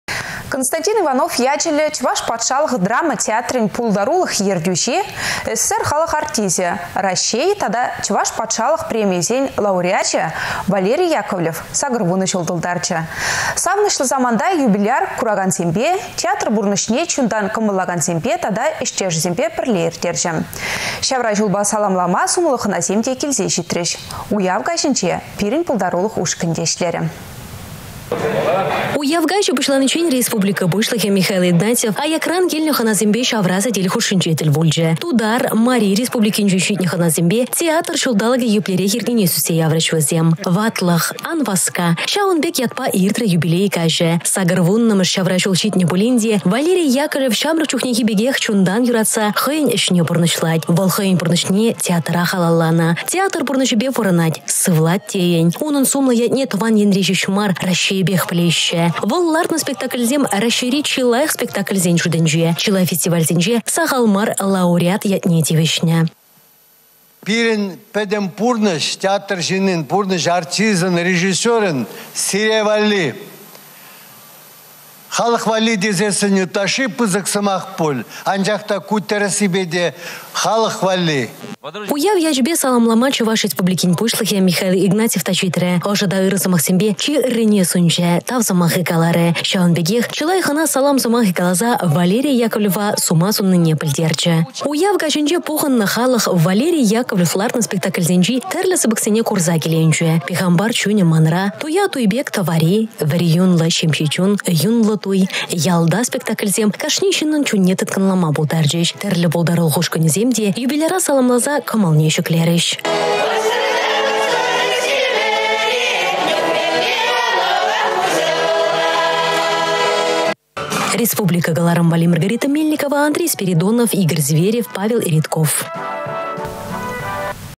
Валерий Яковлев 80 çул тултарчĕ
К.В.Иванов ячĕллĕ Чăваш патшалăх драма театрĕн пултарулăх ертÿçи, СССР халăх артисчĕ, Раççей тата Чăваш Патшалăх премийĕсен лауреачĕ Валерий Яковлев  80 çул тултарчĕ. Савăнăçлă саманта юбиляр куракансемпе, театр пурнăçне чунтан кăмăллакансемпе тата ĕçтешĕсемпе пĕрле ирттерчĕ. Çавра çулпа саламлама сумлă хăнасем те килсе çитрĕç. Уяв каçĕнче пирĕн пултарулăх ушканĕ те пулчĕ.